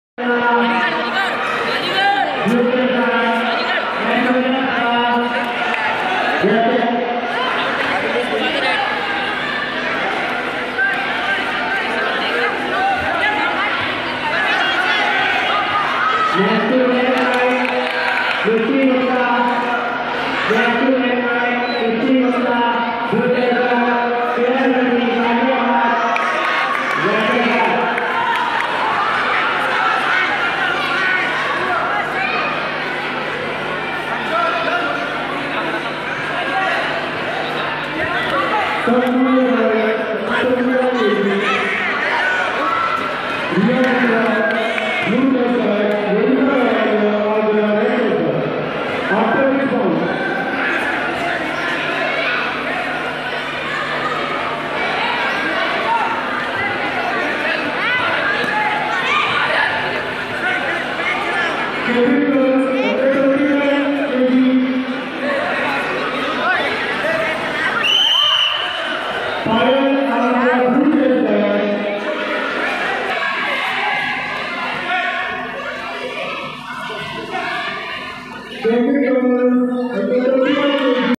Kartik Verma 🔵 UP State Taekwondo Championship Agra, Cadet U-41 kg (1)
Kartik Verma 🔵 UP State Taekwondo Championship Agra, Cadet U-41 kg